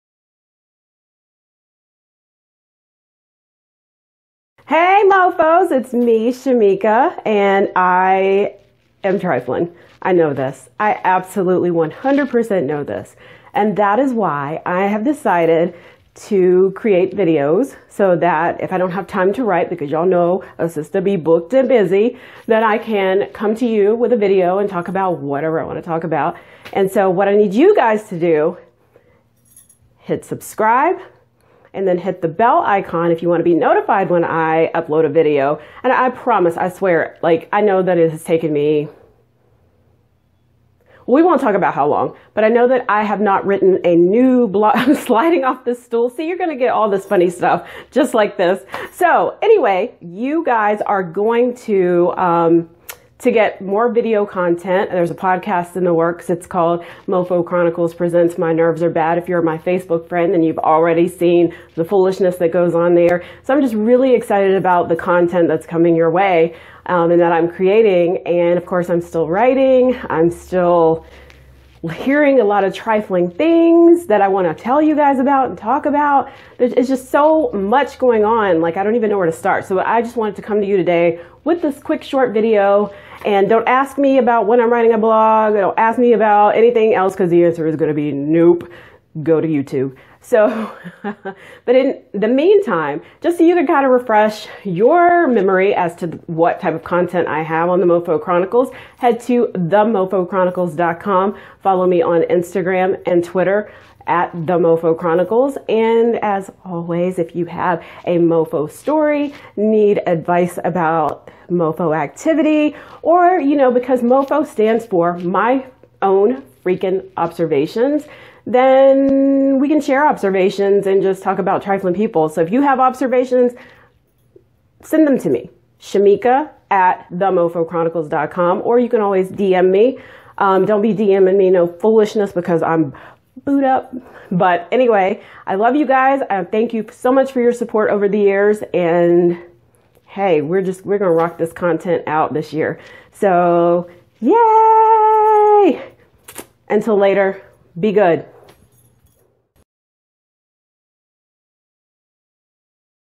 Introducing Mofochronicles: The VLOG EDITION
Hey Mofos! I'm baaaack! My nerves are still bad and now you just get to witness on camera.

The Mofo Chronicles is a place to talk about all the things that I see from MY OWN FREAKIN' OBSERVATIONS. Check out Themofochronicles.com for the blog!